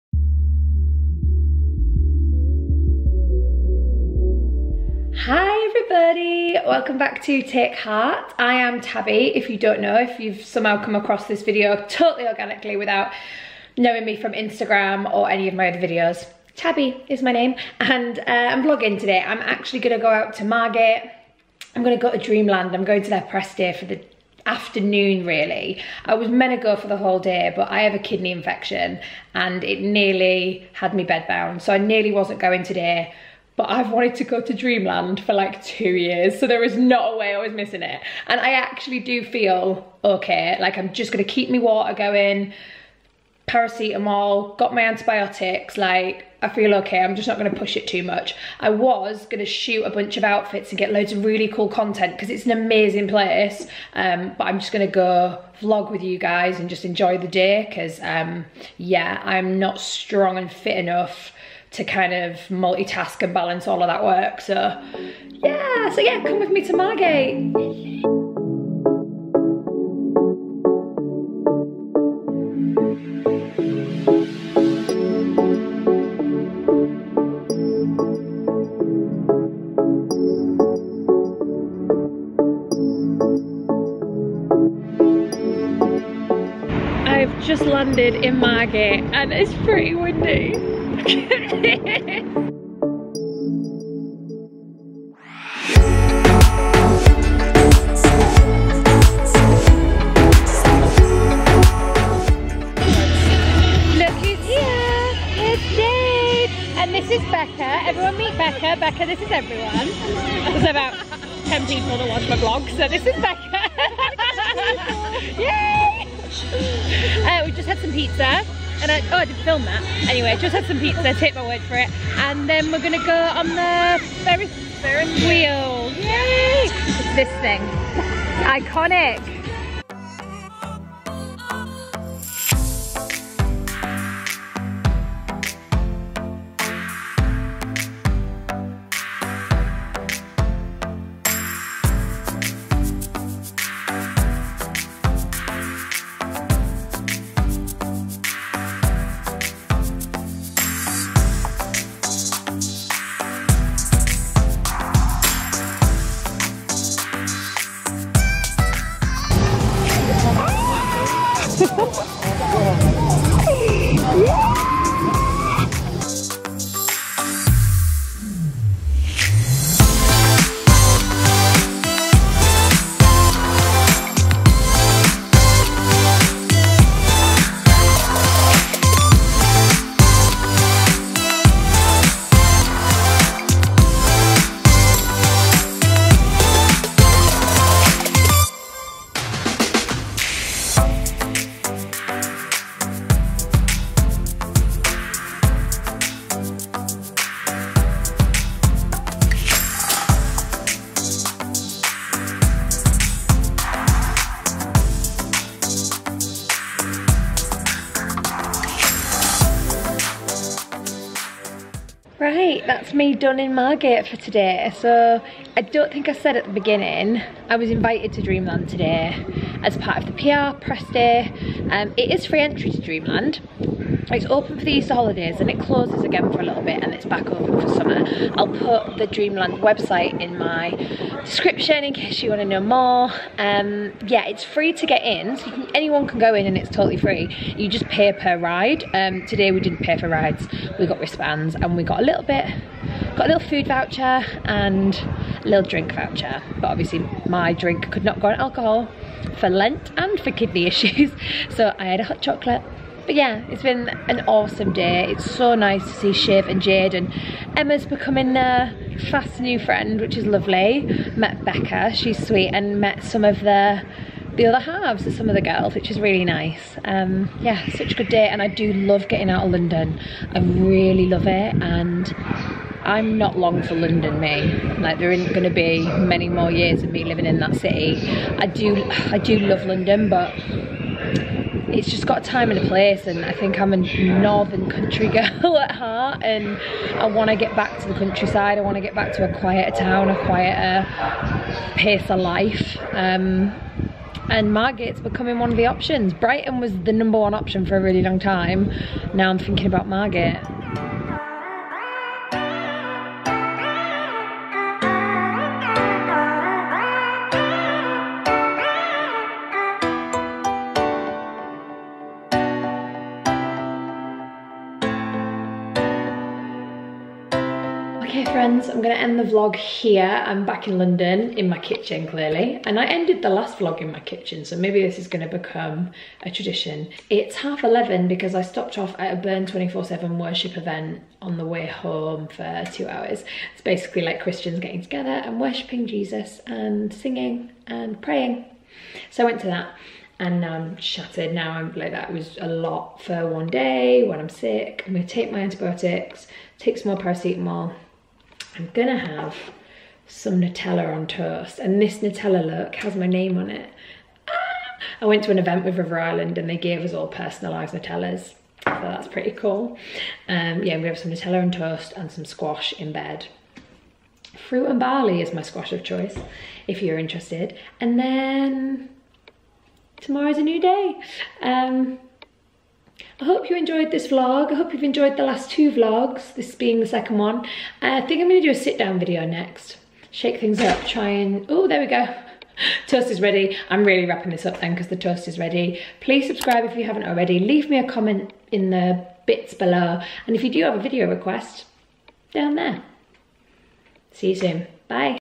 A DAY AT DREAMLAND | VLOG ARCHIVE
I was invited to the Dreamland Press Day at the start of the season to share with you lovely lot what a great day out it is!

[All rides & food included in this video were part of a press package]